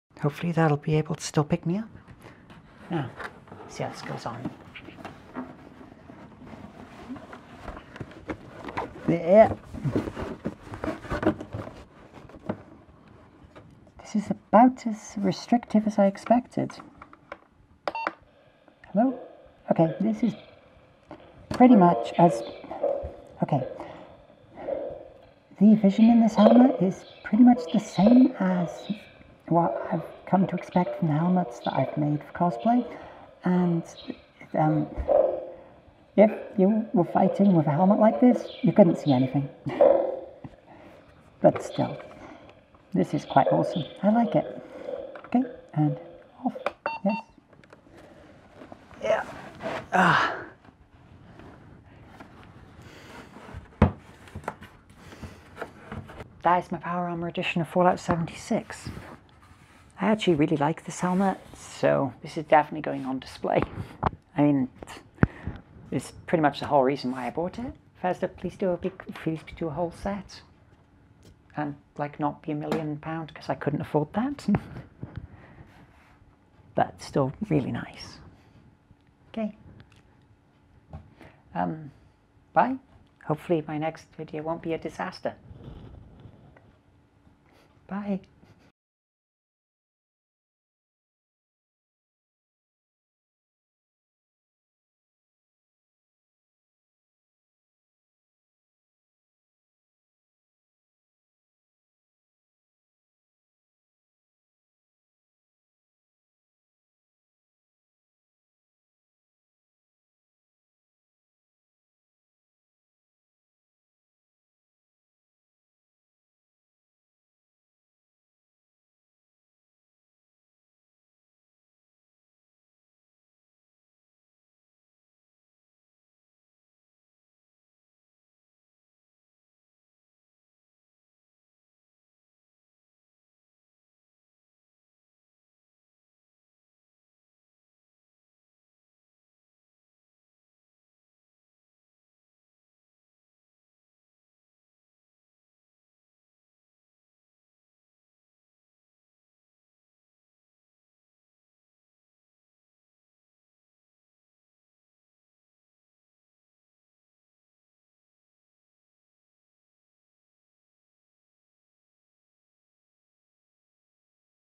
Fallout 76 Power Armour Edition Unboxing
i finally put together my video for unboxing fallout 76!!!
feel free to play it but, i wont be i actually ended up getting headaches playing so yeah, love the helmet tho.